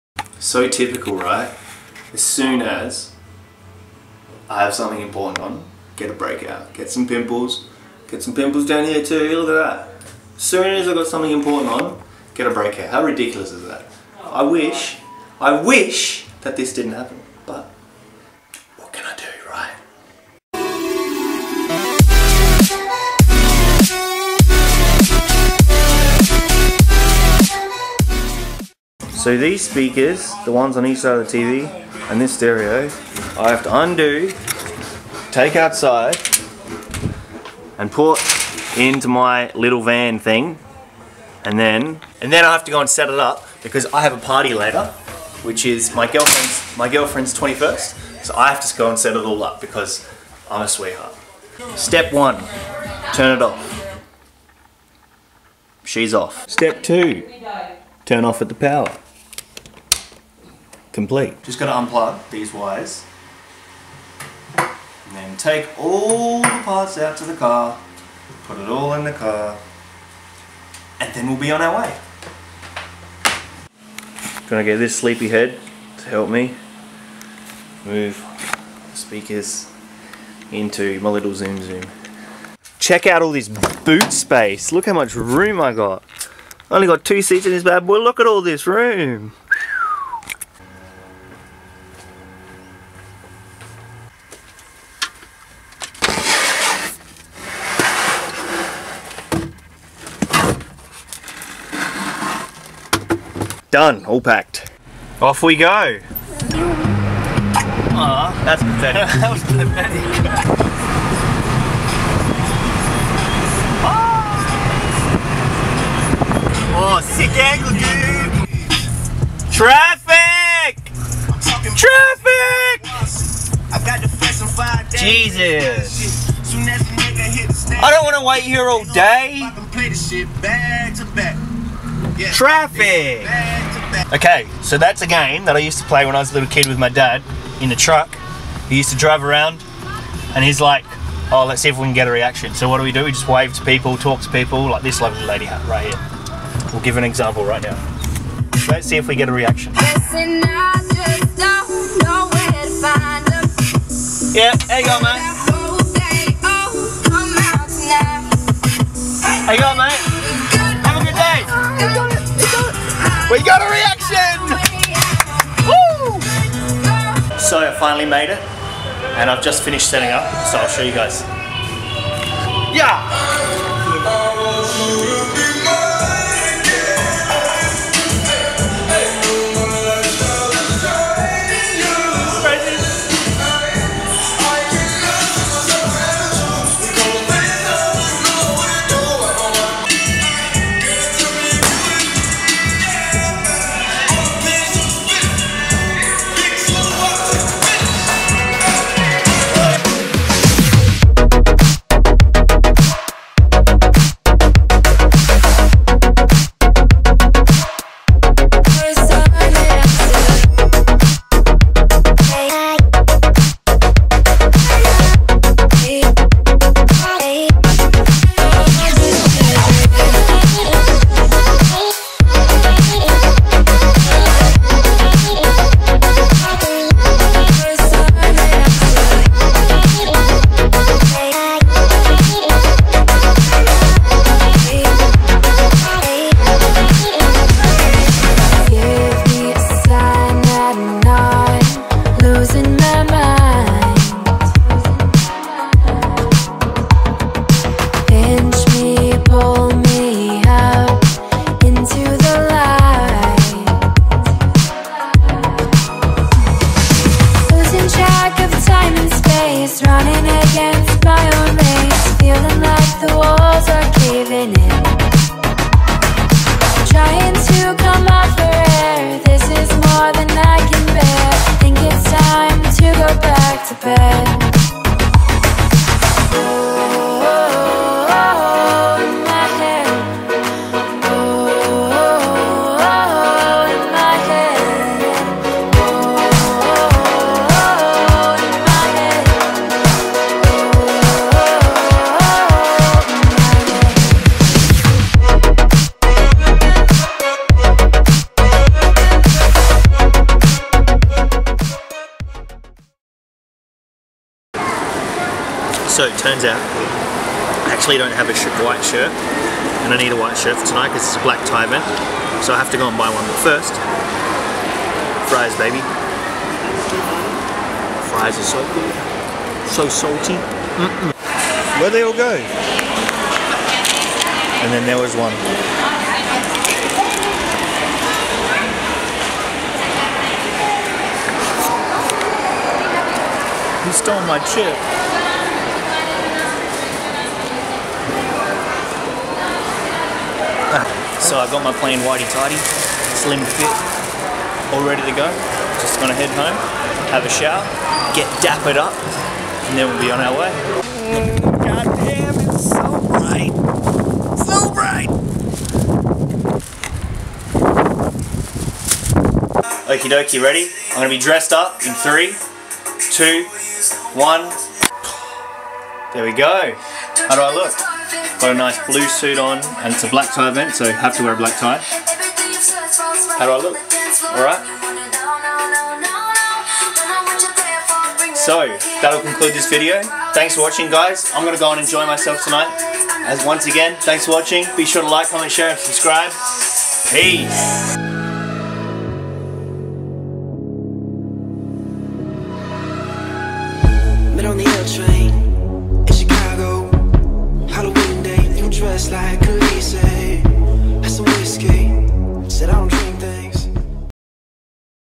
It's Party Time!!!
So after a long period of planning and preparing, I finally got to celebrate my girlfriend's 21st Birthday!

use code "GARGULA15"

MyFitnessPal: WilliamONEfitness

- WilliamONE